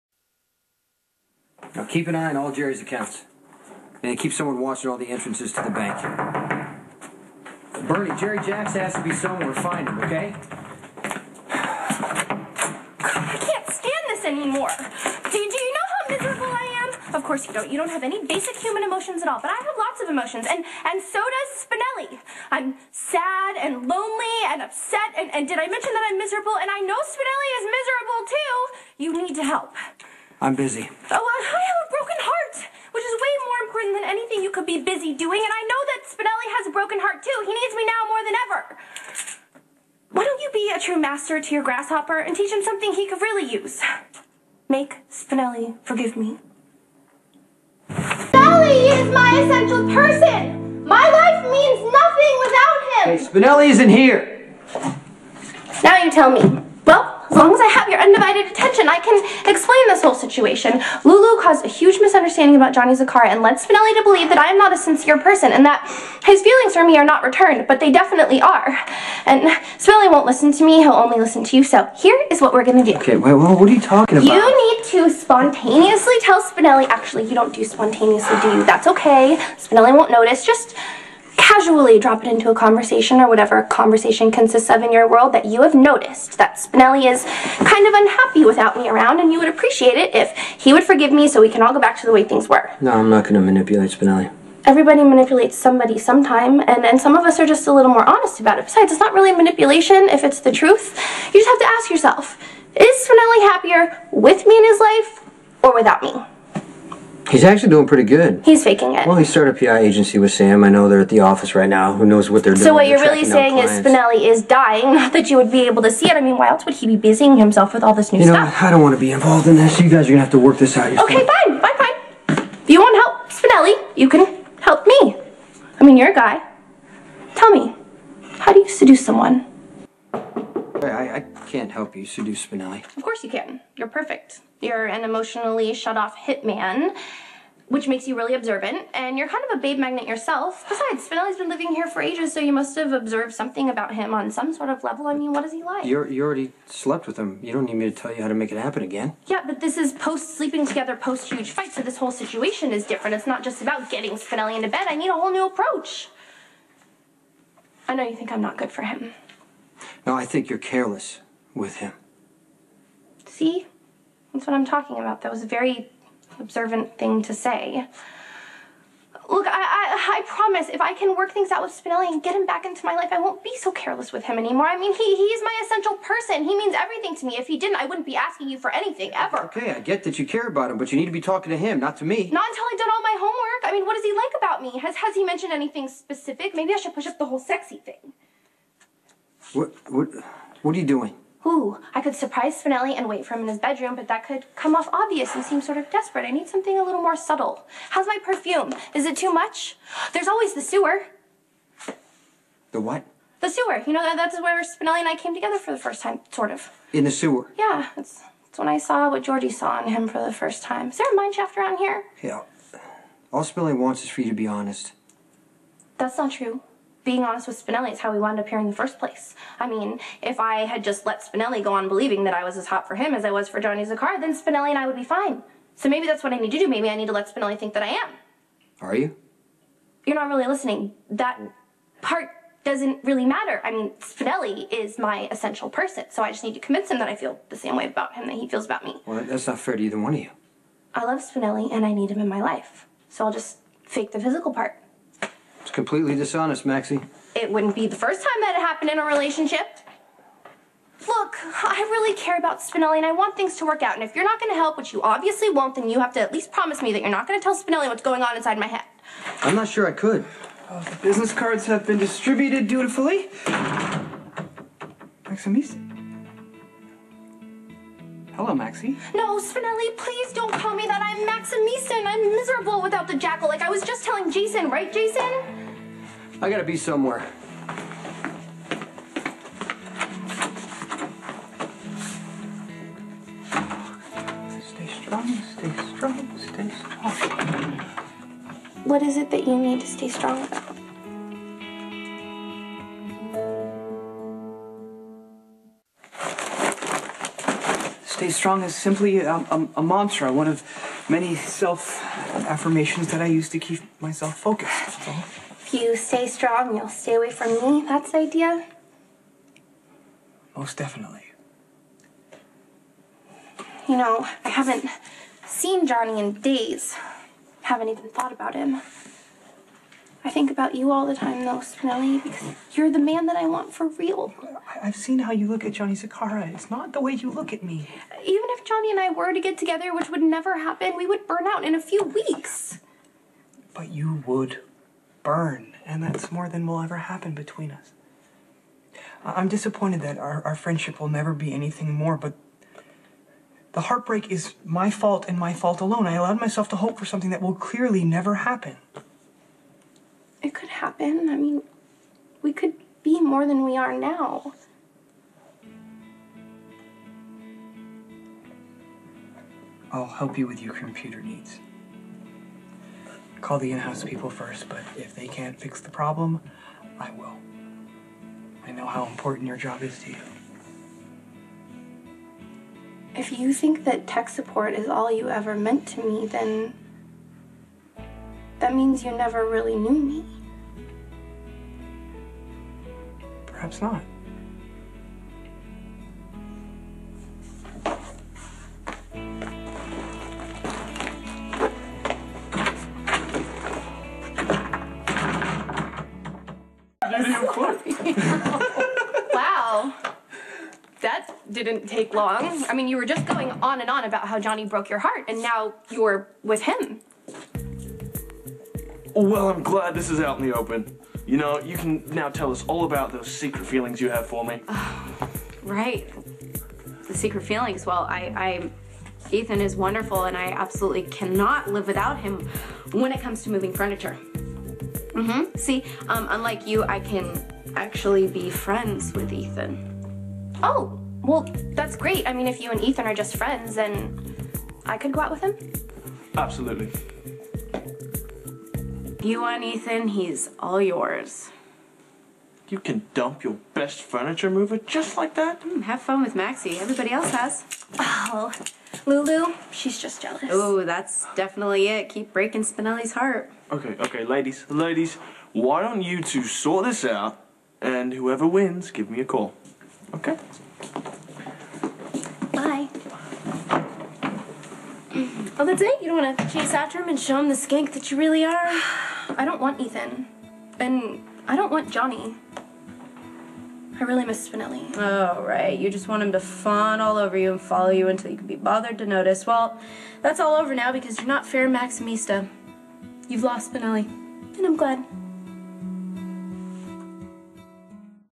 Maxie 4-22-09 *Maxie goes to Jason for help in getting Spinelli back*
Maxie goes to Jason's penthouse and tells him he has to help her with Spinelli. After she starts yelling about how Spinelli needs her, he tells her that Spinelli's not there. Maxie asks Jason to tell Spinelli that he needs her, but he refuses. So Maxie asks him for help on how to seduce Spinelli. Jason won't help her. Spinelli comes home and almost calls her Maximista and Jason takes off. Spinelli tells Maxie that he will only be there if she needs help with her computer, which upsets Maxie and she leaves. When Maxie gets home she find Lulu and Ethan in an awkward position and tells Lulu she moved on fast. They argue and then Maxie asks if Lulu if its ok if she goes on a date with Ethan, to upset Lulu. Ethan agrees, but doesn't want to get in the middle and leaves. Lulu and Maxie bicker.

Credit to Izzy at something-more